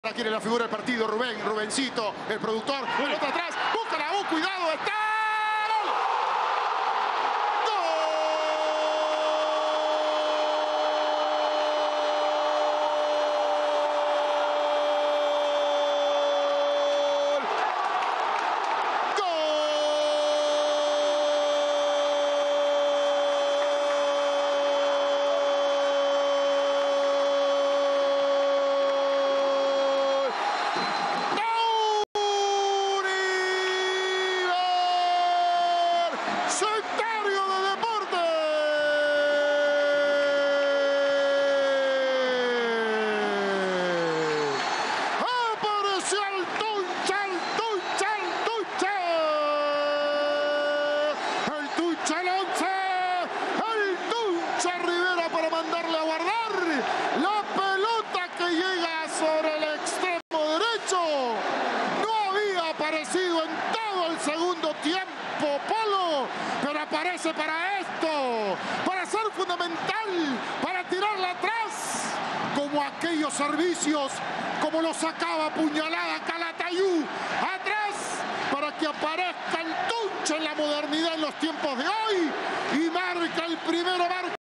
Torneo Apertura: José Rivera puso el Universitario 1-0 Atlético Grau
¡EL TUNCHE FUNDAMENTAL! José Rivera ⚽ marcó el 1-0 de Universitario 🟠 ante Atlético Grau 🔴🟡. El estadio Monumental 🏟 fue una fiesta después del tanto crema.